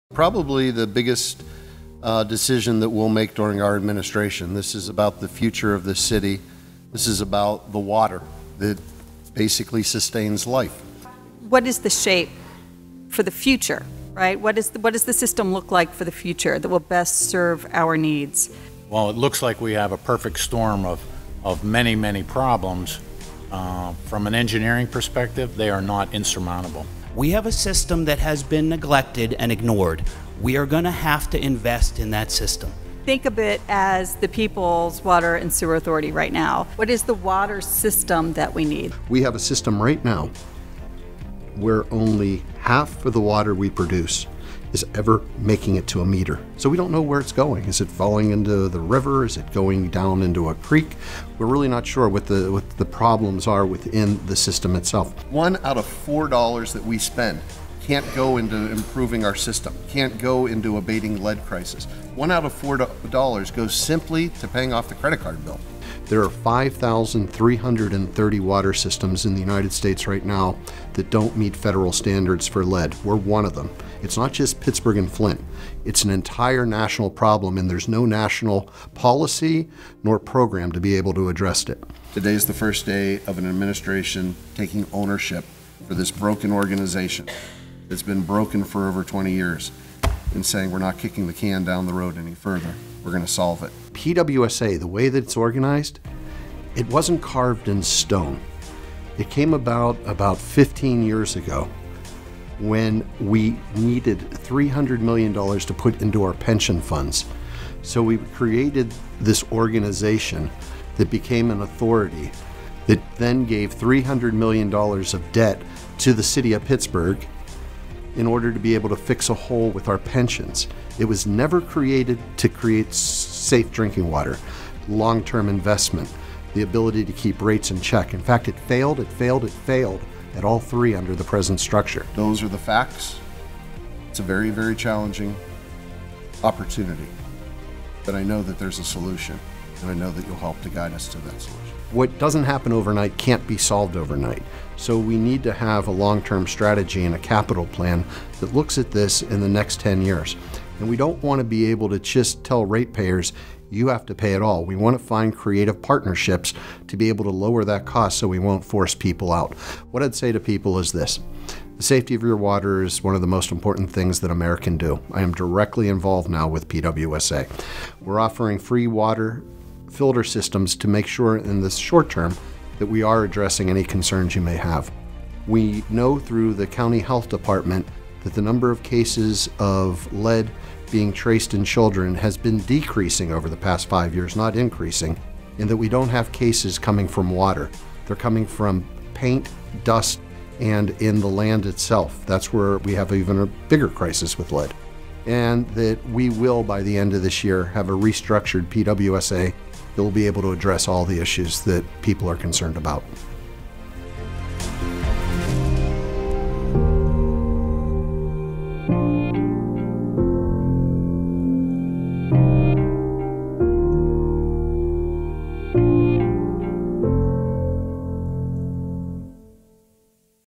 Bill Peduto: Confronting Our Water Challenges Head On
Series of Interviews for the 2017 campaign to re-elect Mayor William Peduto of Pittsburgh.

Mayor Peduto discusses efforts to make a future riverfront for everyone.